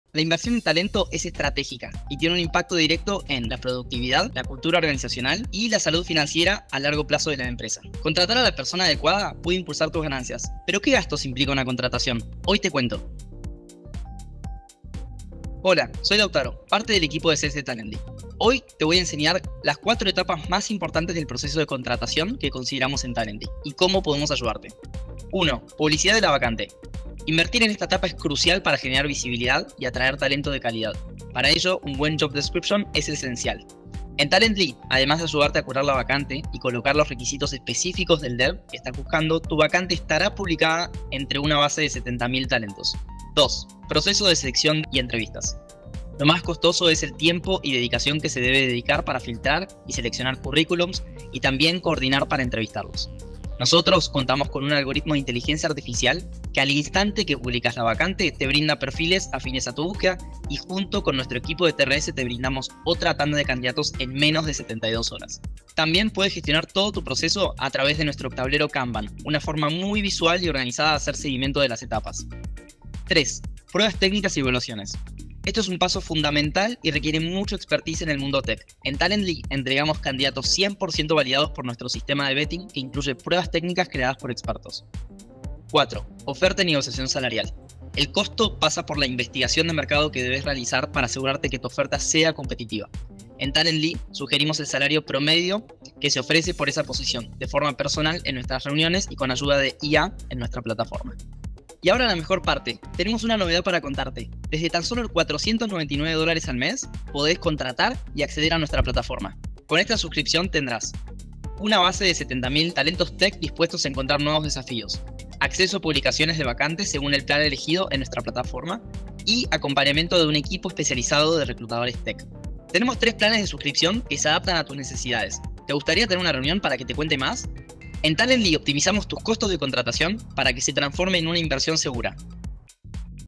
SUSCRIPCIÓN PRO | Optimiza tu inversión con Talently Hire
Tu búsqueda de talento no es un costo, es una inversión que debe ser estratégica para aumentar tus ganancias a largo plazo.

Lautaro del equipo de Sales, te explica 4 etapas del proceso de contratación y cómo podemos ser tu aliado para convertir cada una de ellas en un ahorro de tiempo y dinero.